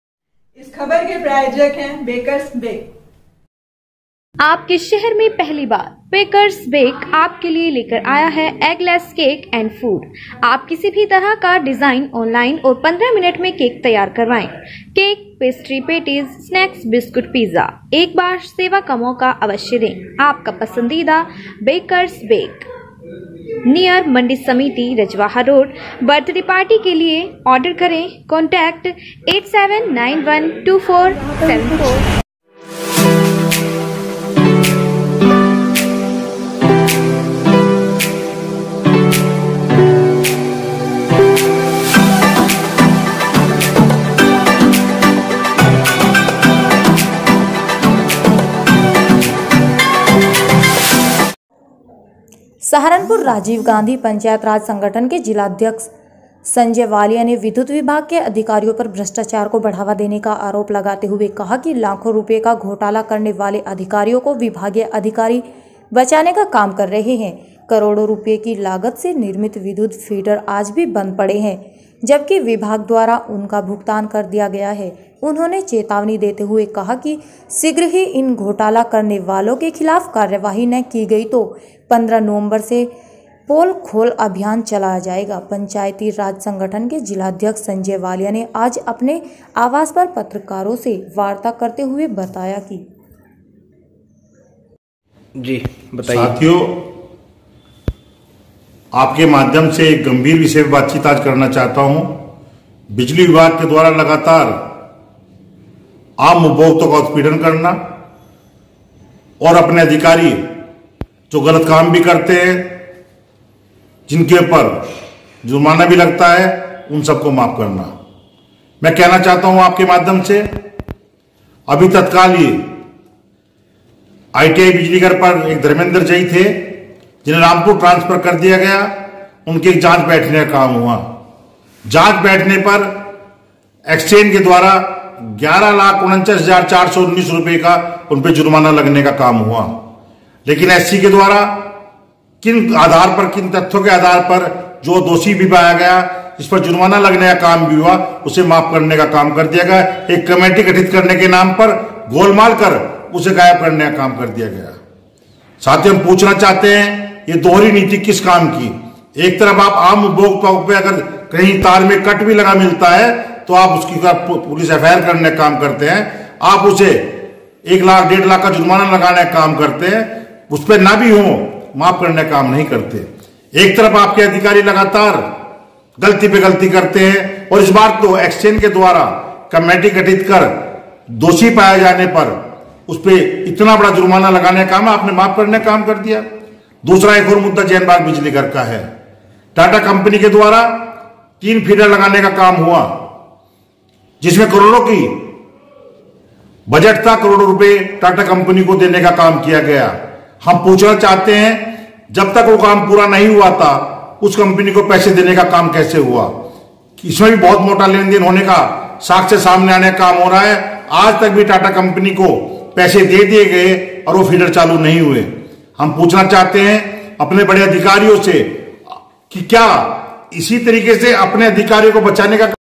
राजीवगांधी पंचायत राज संगठन ने विधुत विभाग के अधिकारियों पर भ्रष्टाचार को बढ़ावा देने का लगाया आरोप
सहारनपुर राजीव गांधी पंचायत राज संगठन के जिला अध्यक्ष ने विधुत विभाग के अधिकारियों पर भ्रष्टाचार को बढ़ावा देने का लगाया आरोप.

एंकर:-सहारनपुर राजीव गांधी पंचायत राज संगठन के जिला अध्यक्ष संजय वालिया ने विद्युत विभाग के अधिकारियों पर भ्रष्टाचार को बढ़ावा देने का आरोप लगाते हुए कहा कि लाखों रुपए का घोटाला करने वाले अधिकारियों को विभागीय अधिकारी बचाने का काम कर रहे हैं करोड़ों रुपए की लागत से निर्मित विद्युत फीडर आज भी बंद पड़े हैं जबकी विभाग द्वारा उनका भुगतान कर दिया गया है उन्होंने चेतावनी देते हुए कहा कि शीघ्र ही इन घोटालों करने वालों के खिलाफ कार्रवाई न की गई तो 15 नवंबर से पोल खोल अभियान चलाया जाएगा पंचायती राज संगठन के जिलाध्यक्ष संजय वालिया ने आज अपने आवास पर पत्रकारों से वार्ता करते हुवे.बताया की ll

बाईट:--संजय वालिया
           कांग्रेस नेता

विक्रम सिंह वर्मा
जिला प्रभारी
सहारनपुर